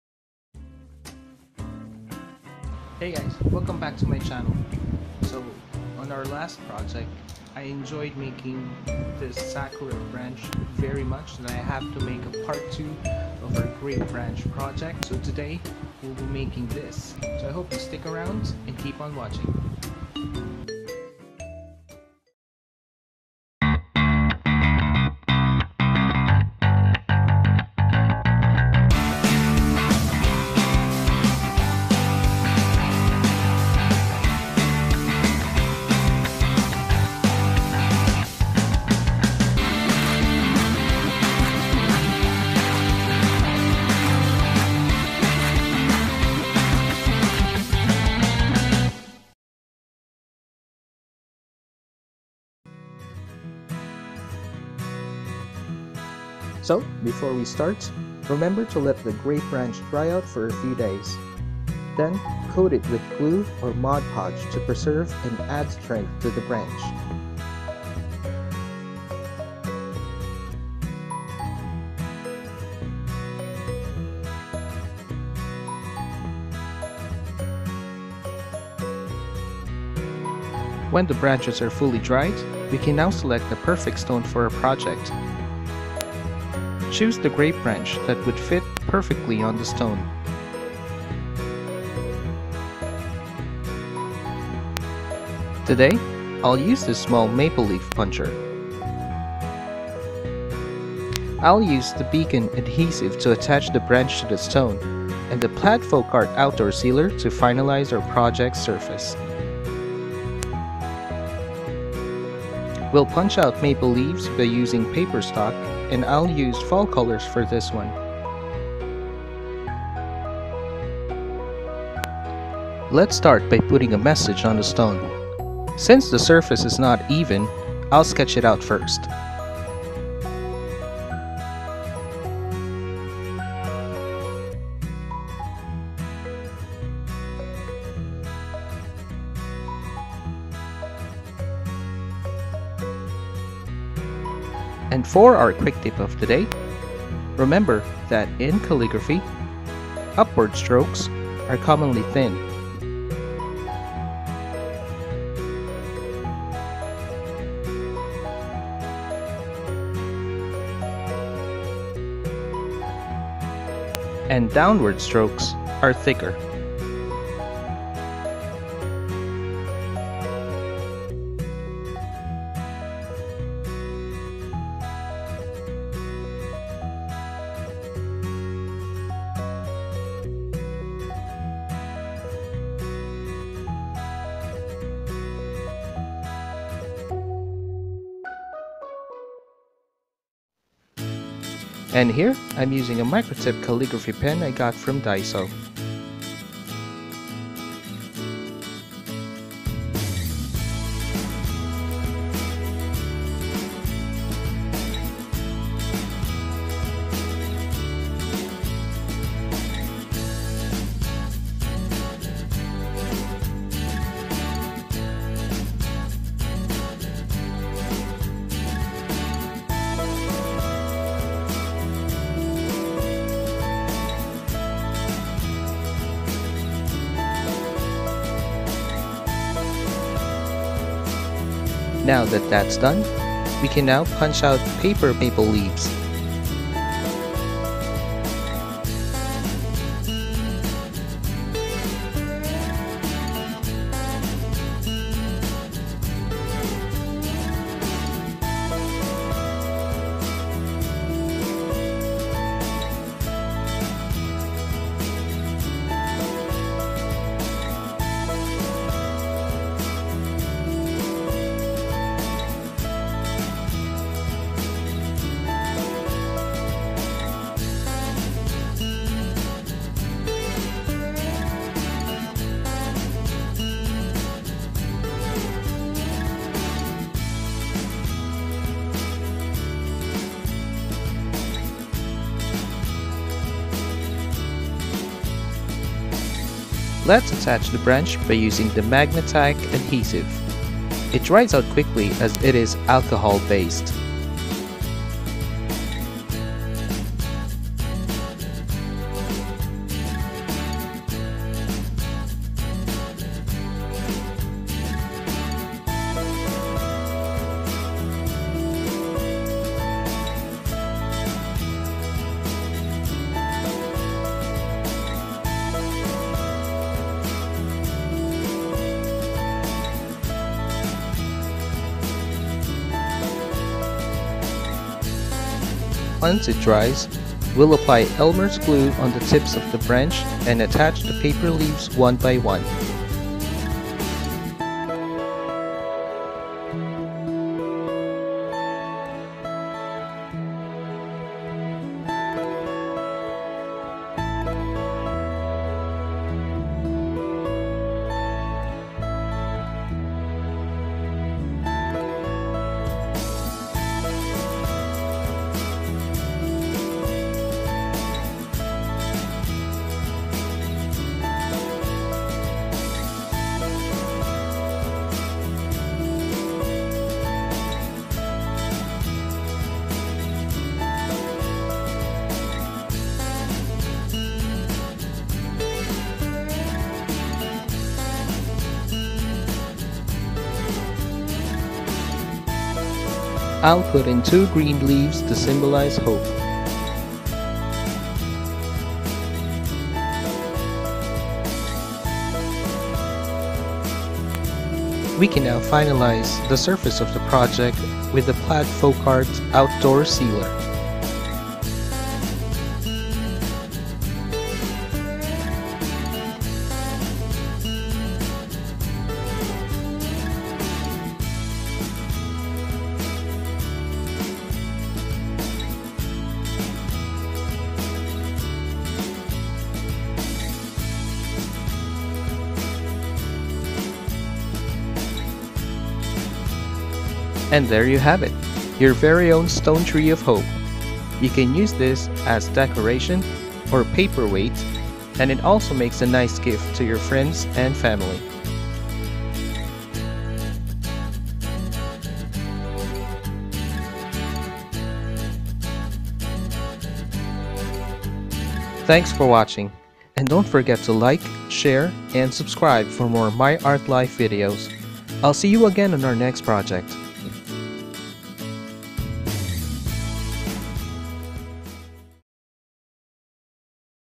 Recycling DESTROYED
Upcycling Project:
Stone Tree: Maple Tree of Hope (Upcycling Grape Branches)

Materials:
Dried Grape Branch
Garden Stone
paper Stock
Elmer's Glue / Mod Podge
Plaid's Folkart Outdoor Satin Sealer
Beacon Magna Tac Permanent Adhesive
Puncher (Leaf Shape)
Marker (for writing wordings on the stone)

Music I used in My Video:
Owl papercraft model from:
Canon Paper Park
Thanks!